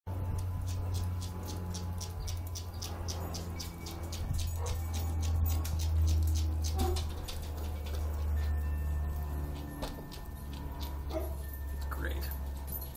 Skunks at dinner time
Our little family of skunks that have been raiding our cat's food. They usually only come at night but they must be hungry.